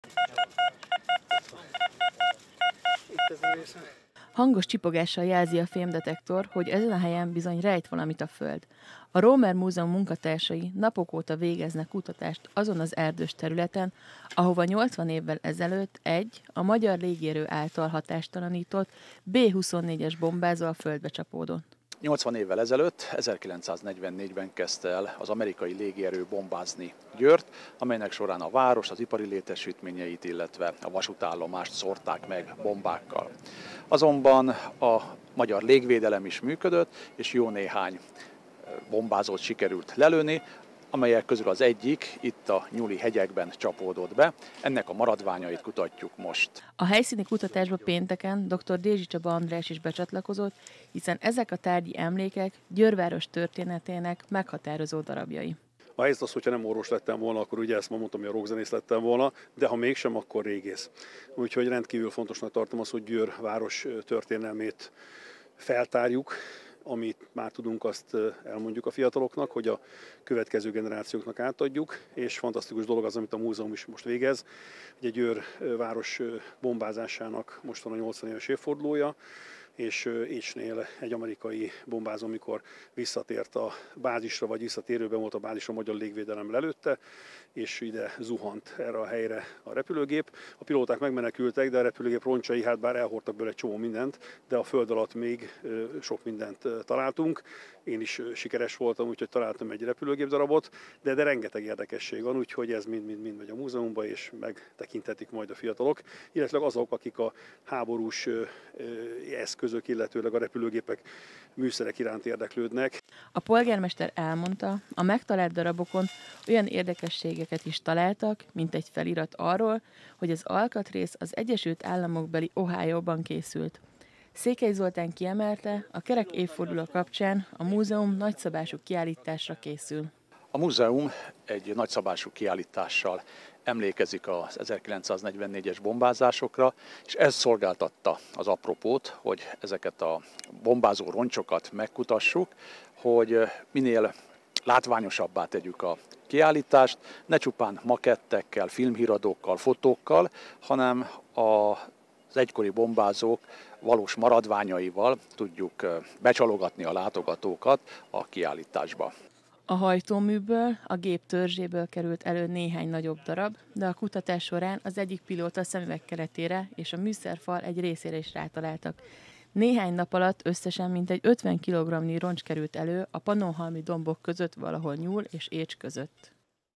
Roncskutatás a dombok között
Idén lesz 80 éve, hogy az angolszász légierő bombatámadást intézett Győr ellen. 1944. április 13-án 11 óra 17 perckor szólaltak meg a légvédelmi szirénák, a Liberátor típusú bombázók több tucat robbanószerkezetet zúdítottak a városra. A magyar légierő több ellenséges repülőt is kilőtt, az egyik a Pannonhalmi-dombságban zuhant le. A Rómer Múzeum munkatársai ennek a B-24-es bombázónak a maradványait kutatják a dombok lankáin. Dr. Dézsi Csaba András polgármester is tevékenyen részt vett a roncskeresésben, hiszen ezek az emlékek, Győr történelmének fontos részletei.